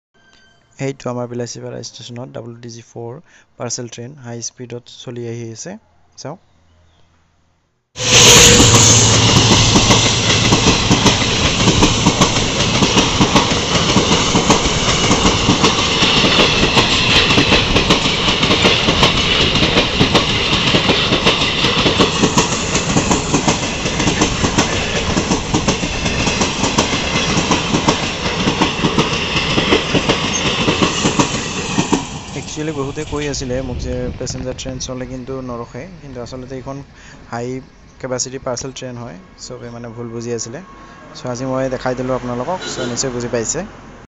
Bilasipara Train Service,,,,High Capacity Parcel Train Located At Bilasipara Train Station
High speed Parcel Train At Bilasipara Train station With most powerful  EMD engine WDG4 G